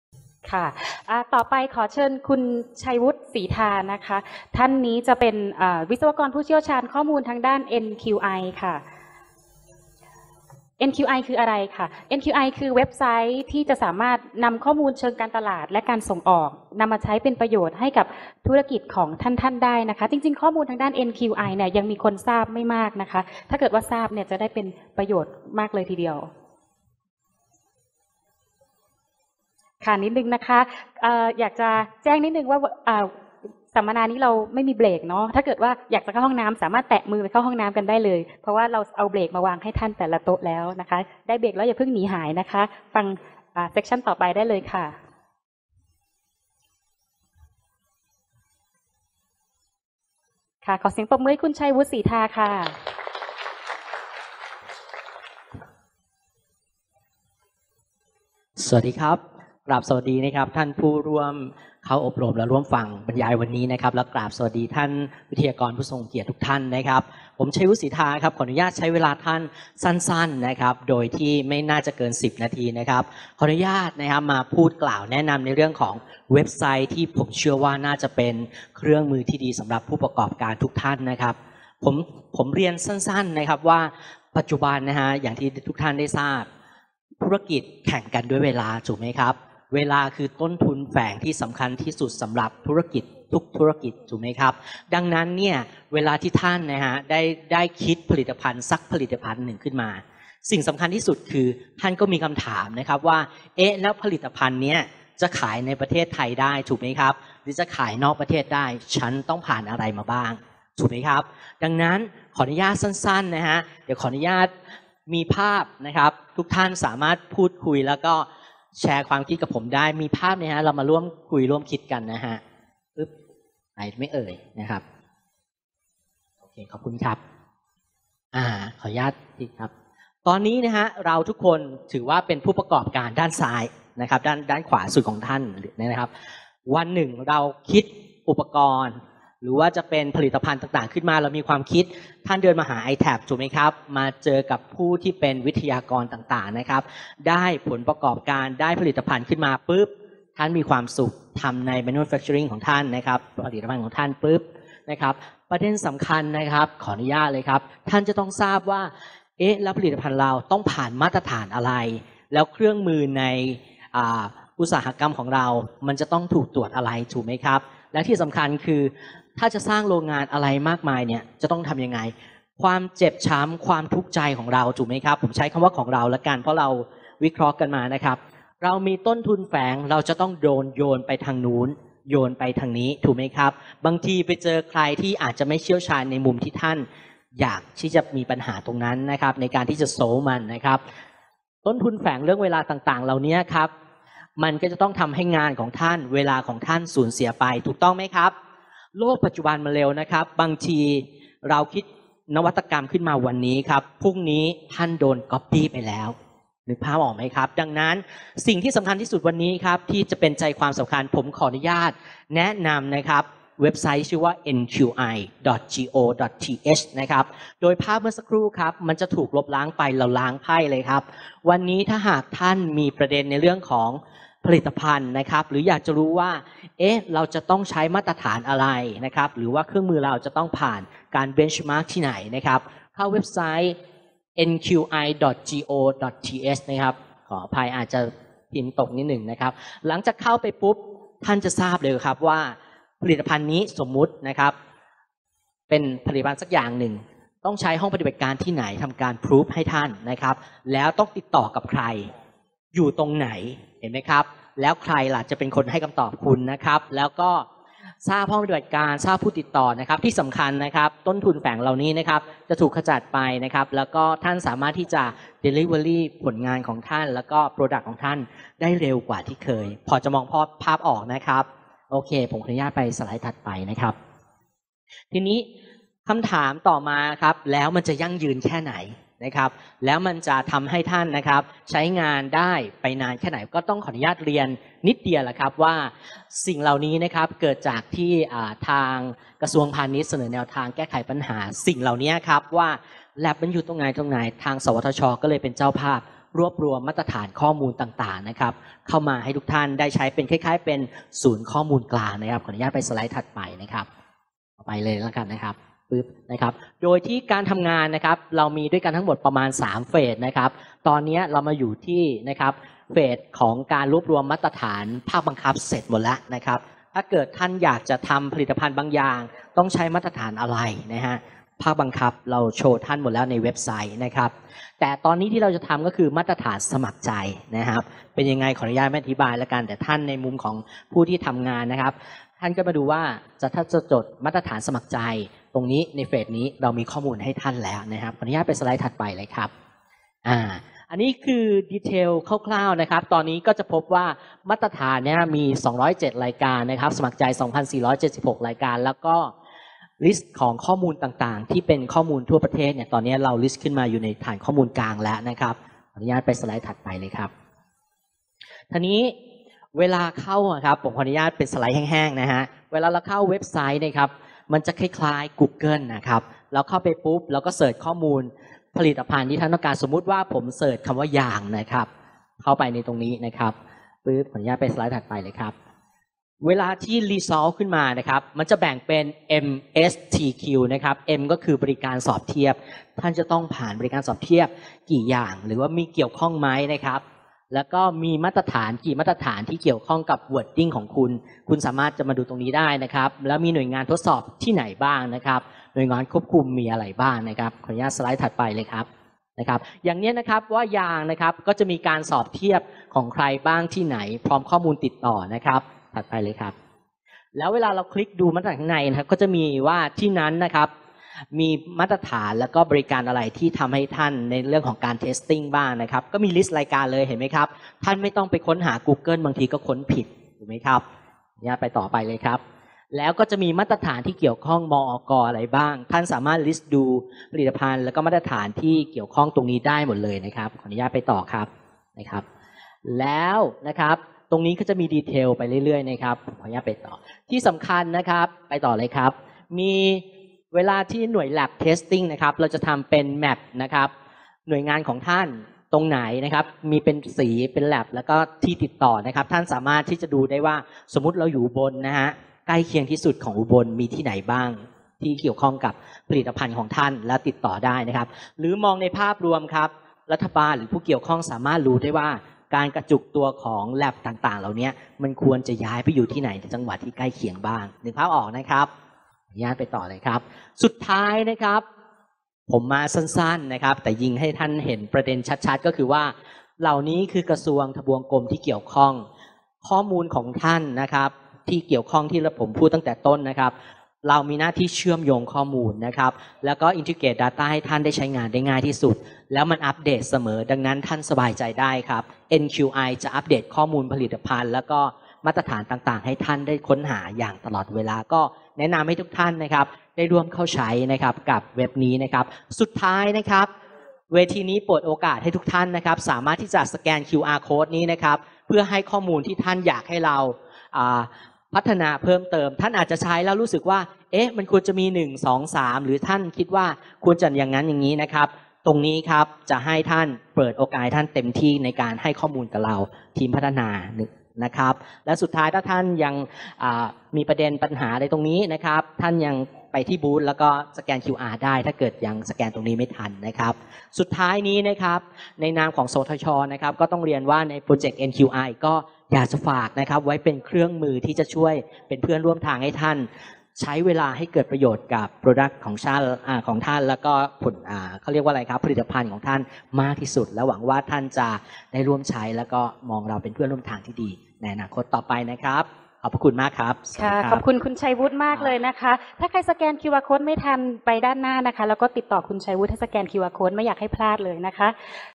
การเสวนา "วิทย์เพื่อธุรกิจ" (3/6)
คุณชัยวุฒิ สีทา วิศวกรผู้เชี่ยวชาญข้อมูล NQI สำนักงานพัฒนาวิทยาศาสตร์และเทคโนโลยีแห่งชาติ (สวทช.) บรรยายพิเศษในหัวข้อ “SME ไทยเข้มแข็งด้วยบริการจากภาครัฐ” การเสวนาในหัวข้อ "วิทย์เพื่อธุรกิจ" ในงาน สวทช.-วิทย์สัญจร “วิจัยเข้มแข็ง เสริมแกร่งภูมิภาค” วันศุกร์ที่ 15 มิถุนายน 2561 เวลา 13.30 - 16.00 น. ณ ห้องประชุมทับทิมสยาม 4 สุนีย์แกรนด์โฮเทล แอนด์ คอนเวนชั่น จังหวัดอุบลราชธานี